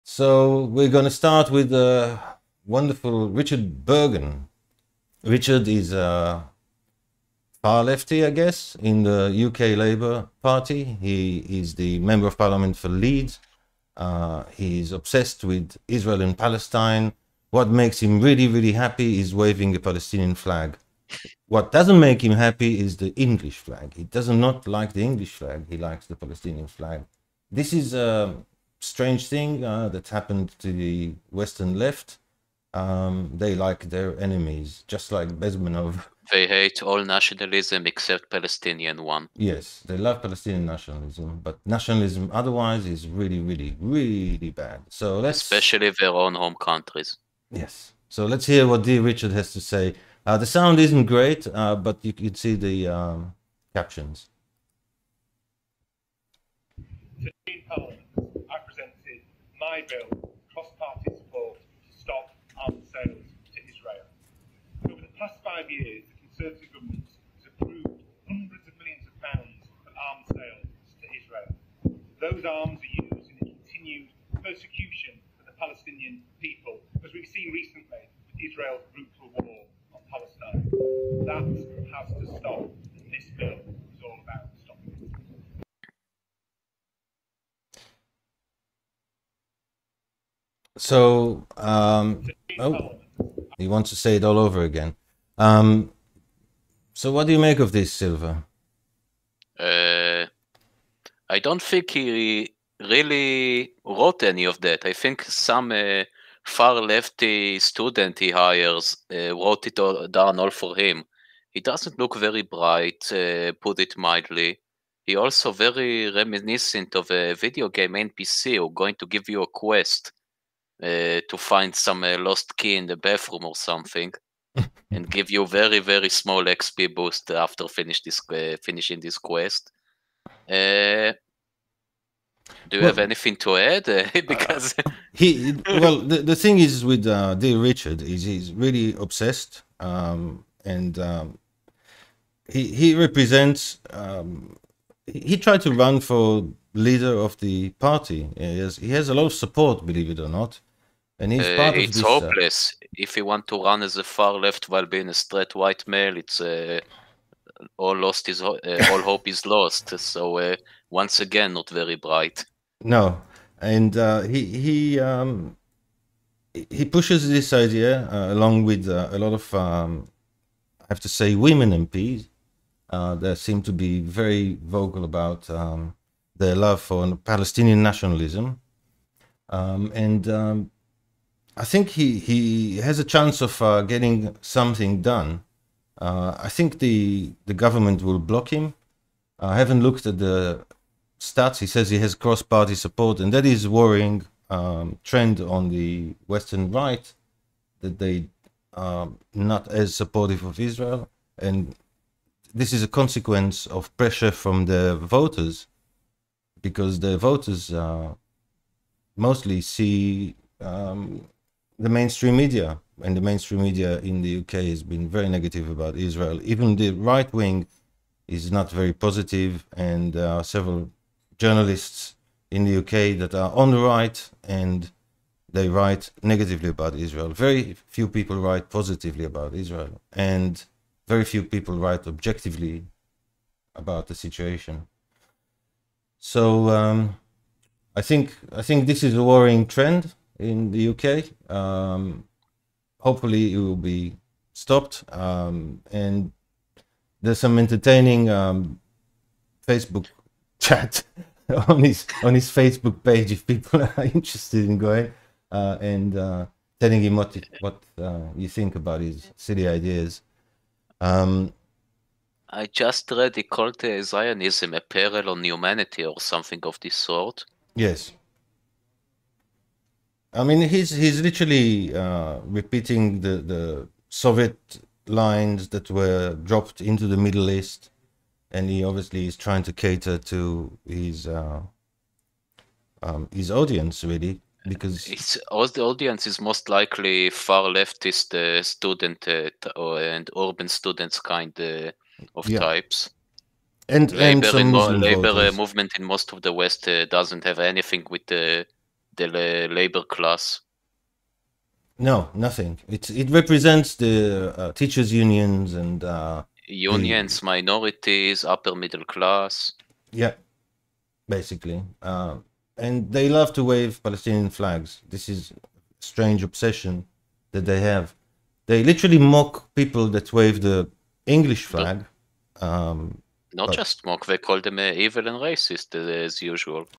Richard Burgon's obsession with Israel
Burgon, a British MP, is famous for worrying about the things his voters don't. This time it's his pet obsession - Israel. The Jewminati investigate.